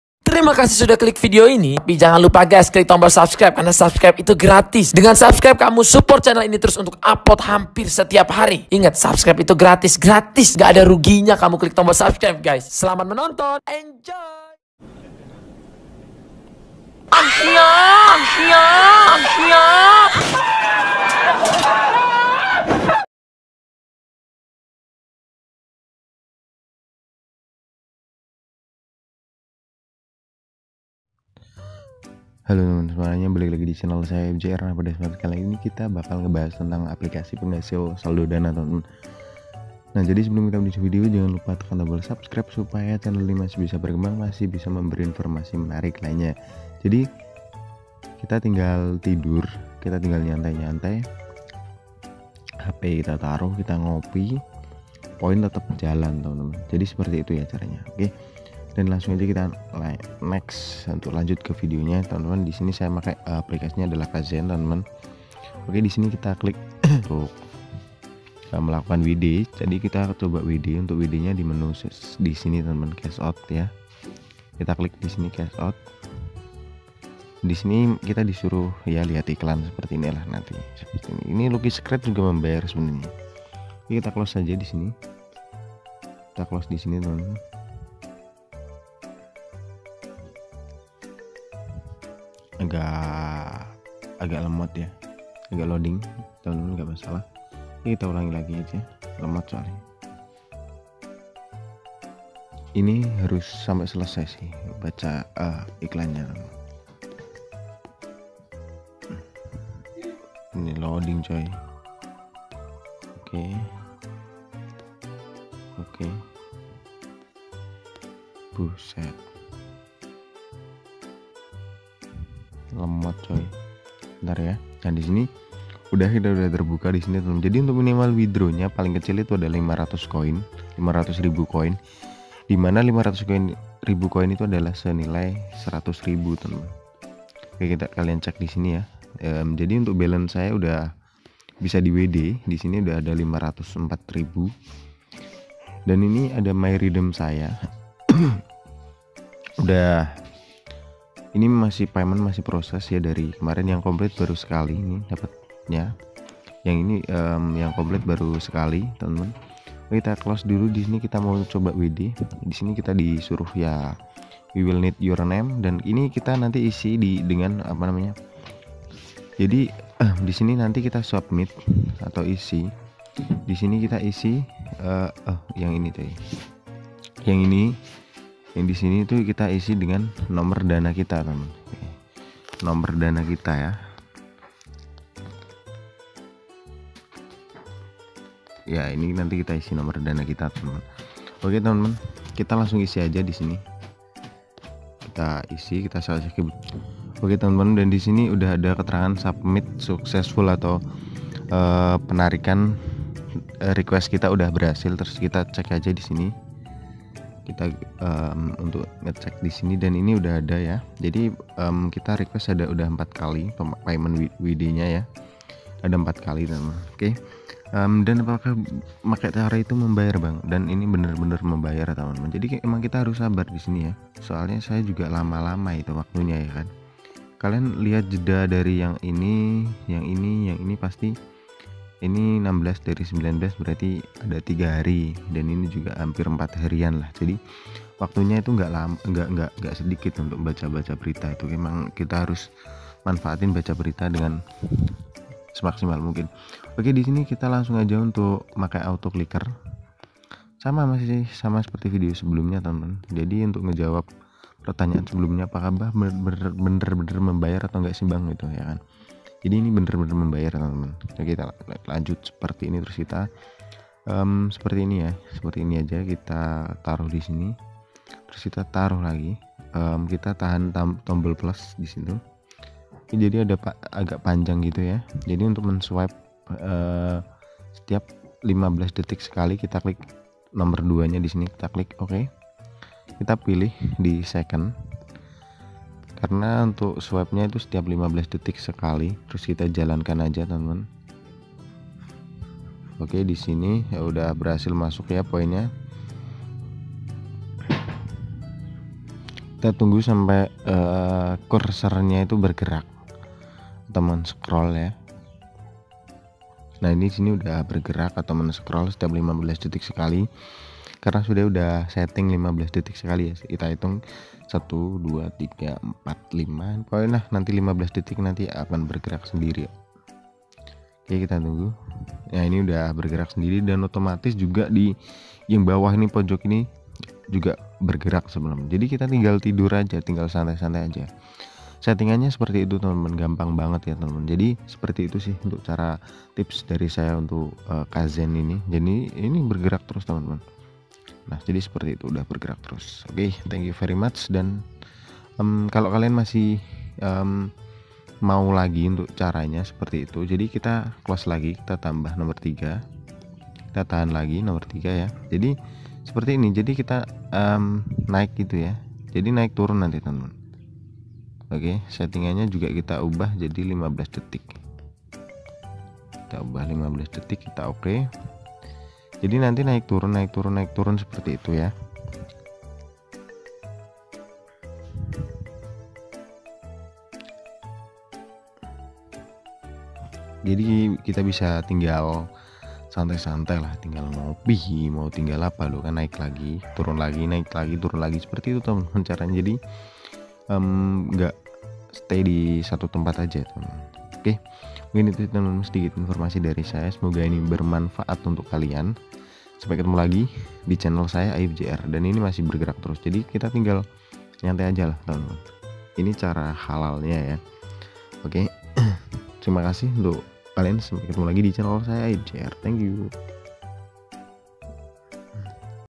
Aplikasi penghasil saldo DANA terbukti membayar 100% + cara cepat
untuk link download apk nya disini